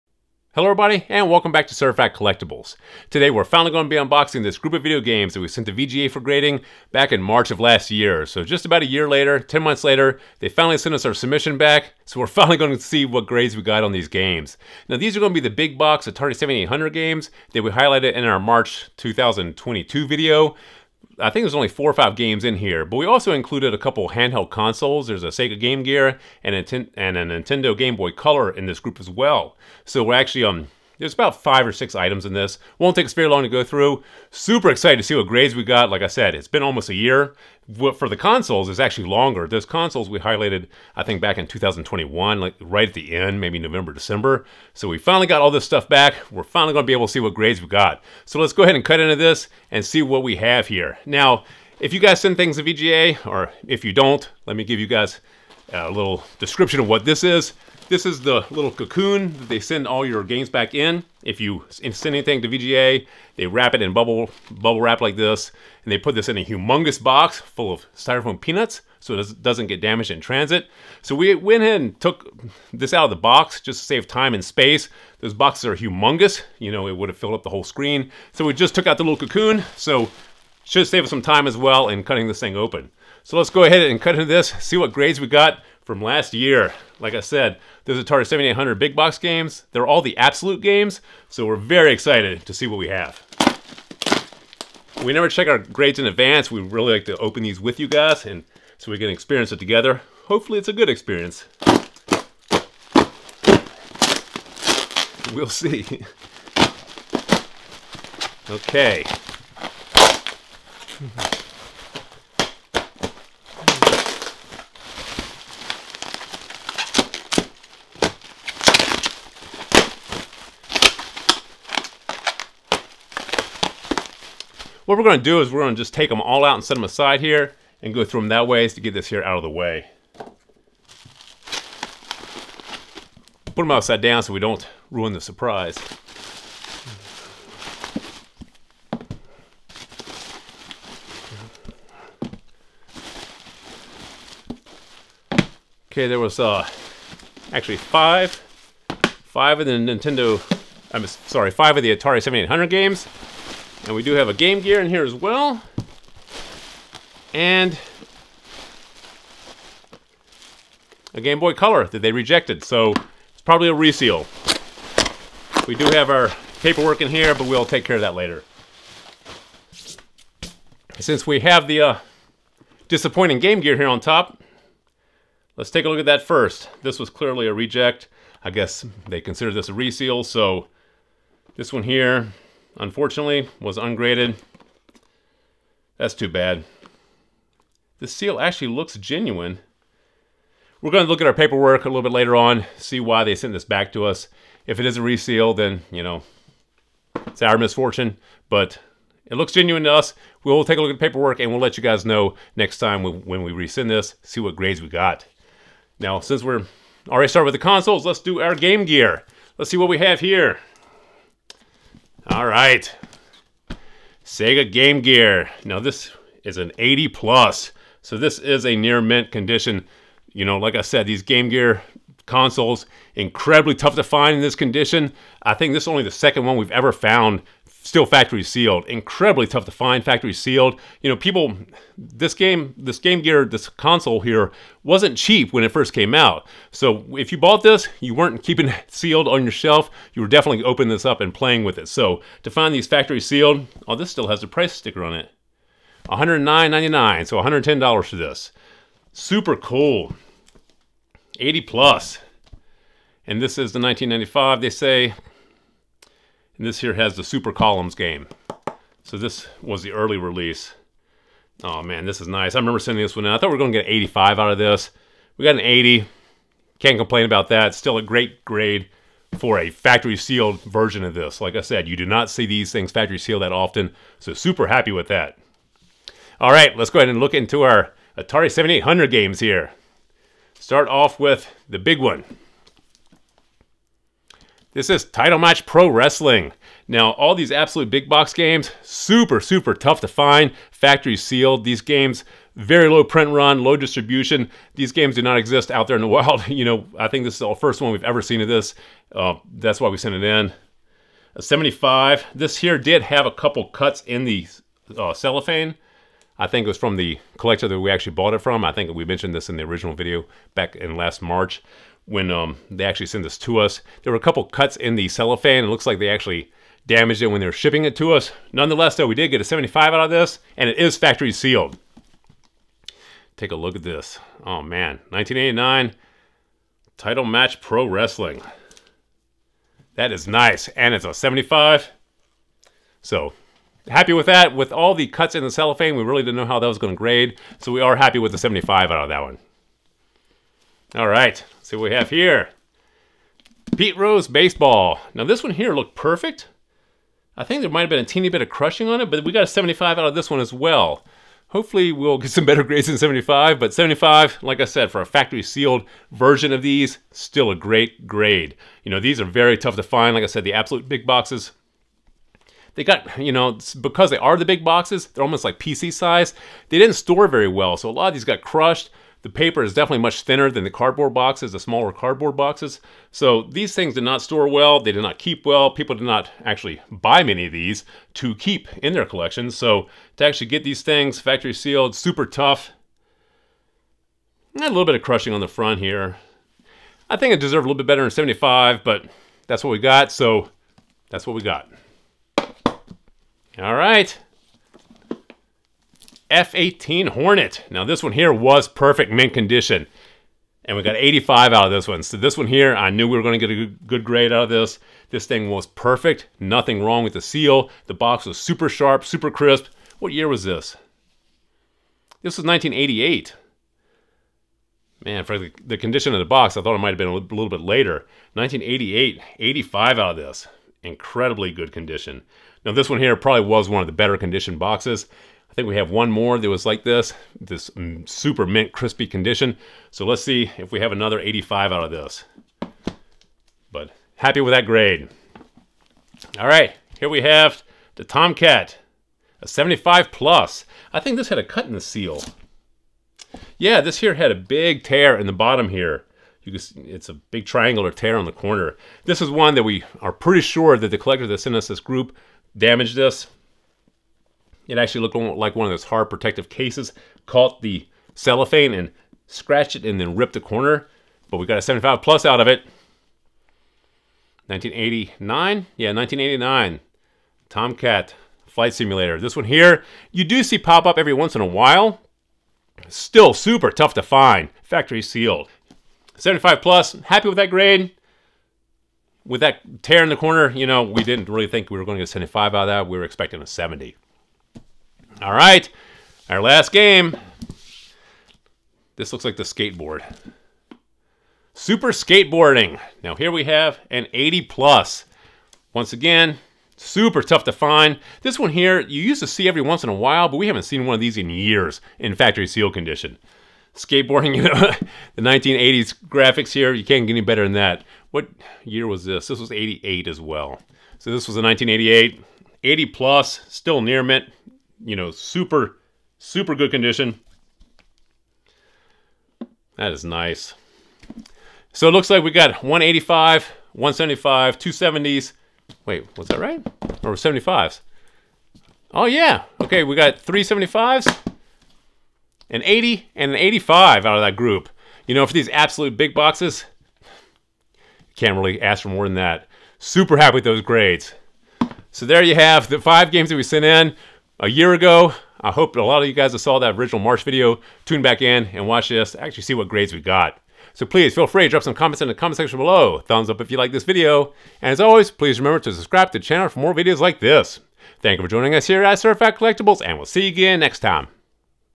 UNBOXING: VGA Graded Video Games and Consoles
In this video we unbox a group of rare, factory-sealed video games for the Atari 7800 and a few handheld consoles, all graded by VGA. This is the follow up video to the one we released on March 20, 2022.